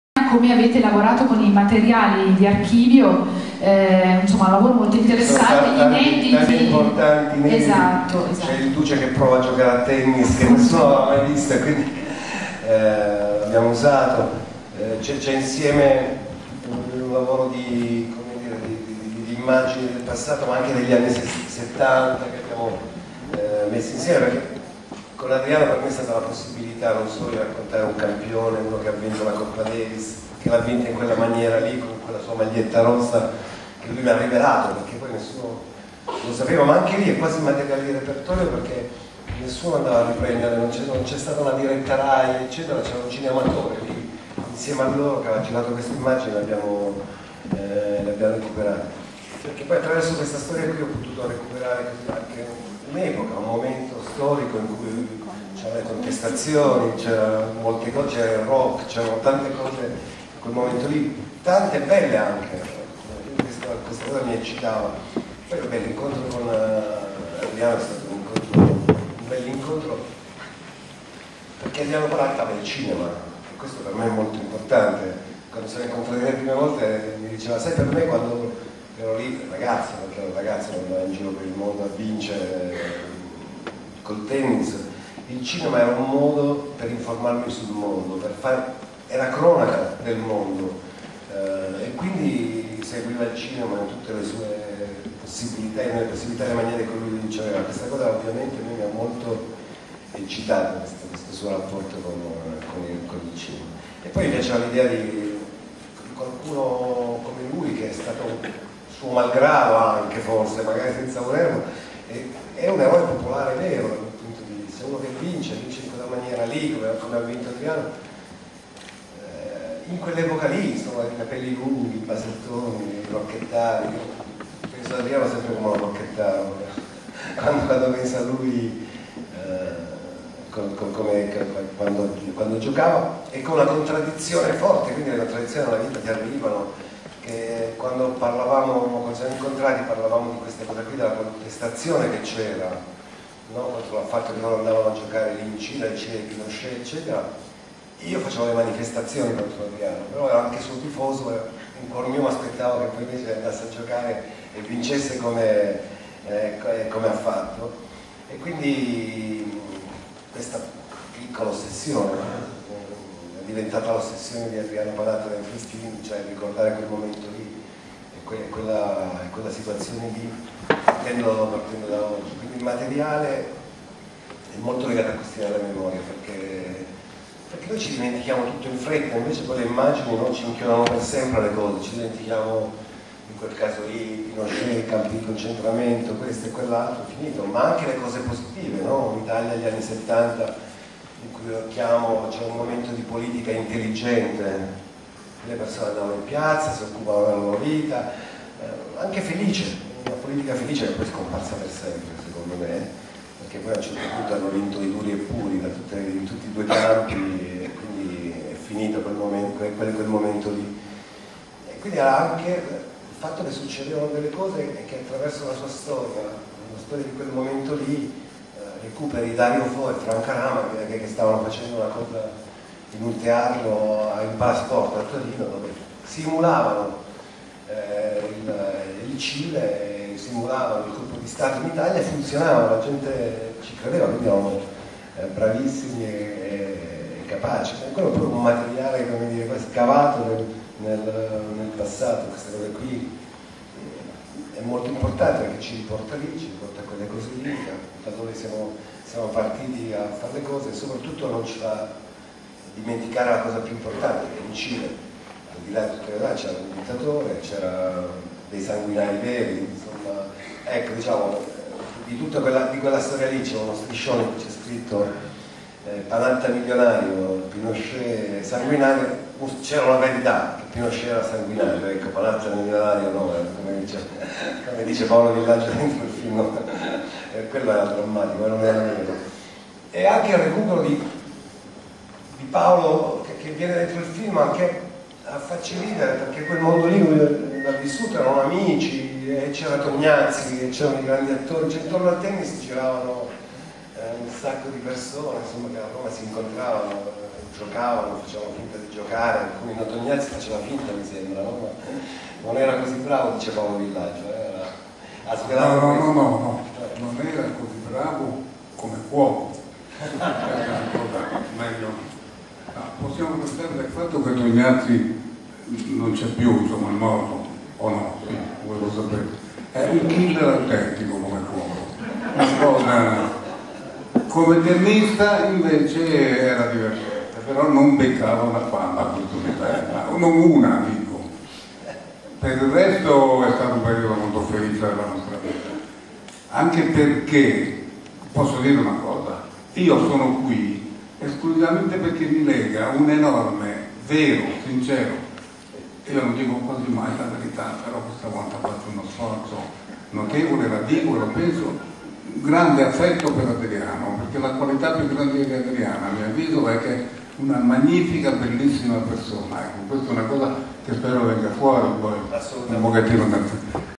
LA MAGLIETTA ROSSA - Mimmo Calopresti, Adriano Panatta e Paolo Villaggio all'Auditorium
Conferenza Stampa al Festival Internazionale del Film di Roma 2009.